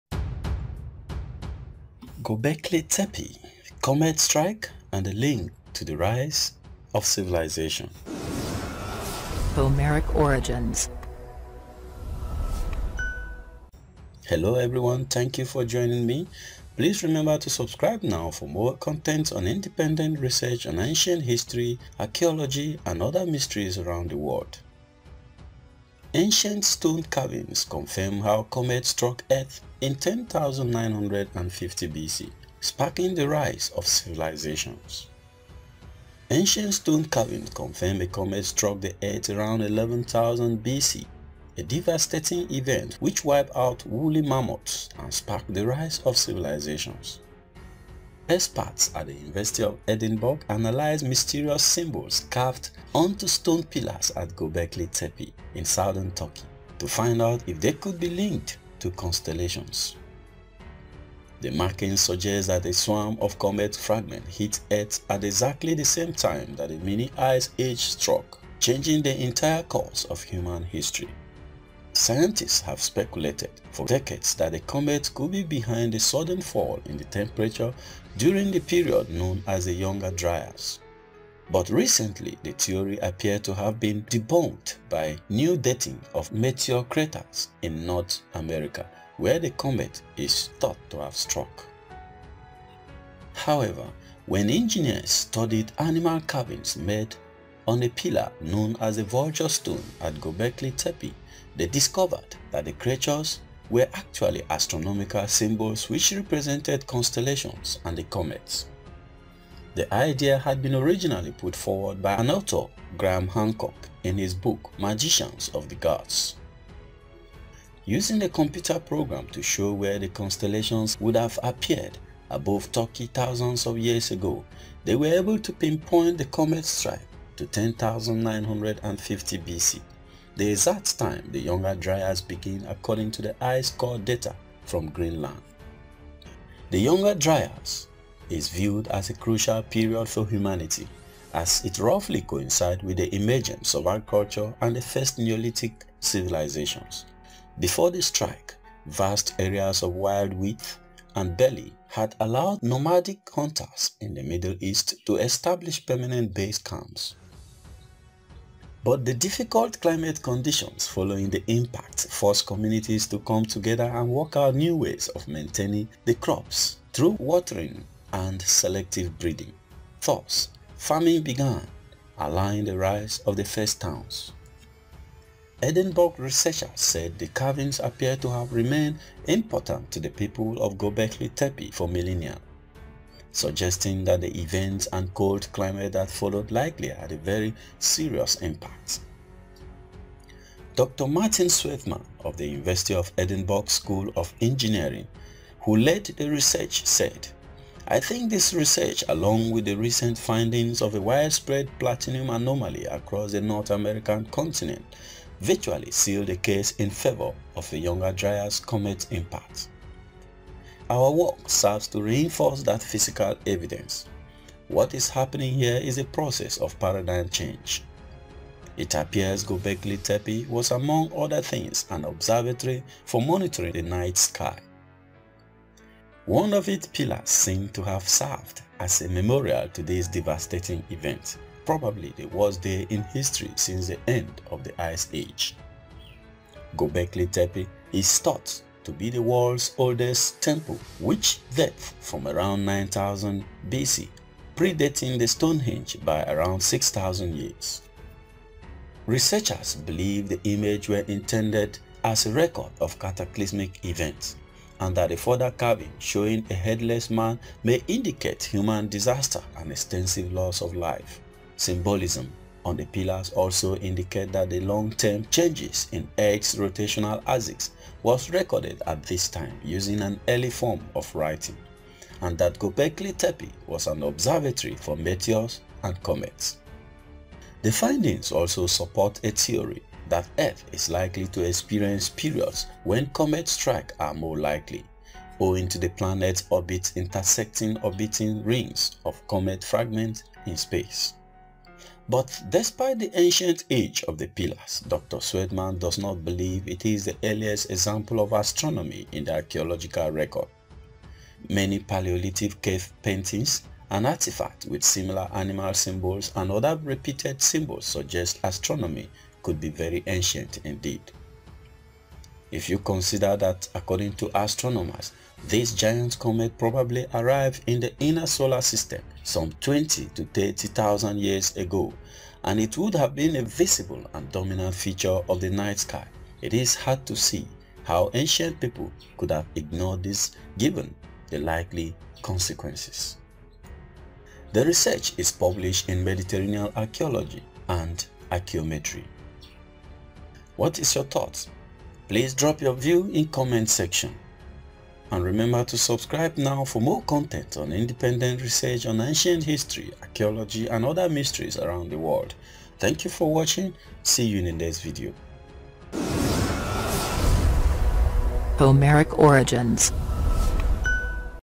Gobekli Tepe - Ancient Stone Carvings, Younger Dryas & Comet Impact Ca. 12,000 BC.
Gobekli Tepe Carvings Confirms Comet Impact 11950 BC. Ancient stone carvings confirm how comet struck Earth in 10,950BC, sparking the rise of civilizations
Ancient stone carvings confirm that a comet struck the Earth around 11,000BC, a devastating event which wiped out woolly mammoths and sparked the rise of civilizations.
Experts at the University of Edinburgh analyzed mysterious symbols carved onto stone pillars at Gobekli Tepe in southern Turkey, to find out if they could be linked to constellations.
The markings suggest that a swarm of comet fragments hit Earth at the exact same time that a mini-ice age struck, changing the entire course of human history.
Scientists have speculated for decades that a comet could be behind the sudden fall in temperature during a period known as the Younger Dryas. But recently the theory appeared to have been debunked by new dating of meteor craters in North America where the comet is thought to have struck.
However, when engineers studied animal carvings made on a pillar – known as the vulture stone – at Gobekli Tepe they discovered that the creatures were actually astronomical symbols which represented constellations and the comet.
The idea had been originally put forward by author Graham Hancock in his book Magicians of the Gods.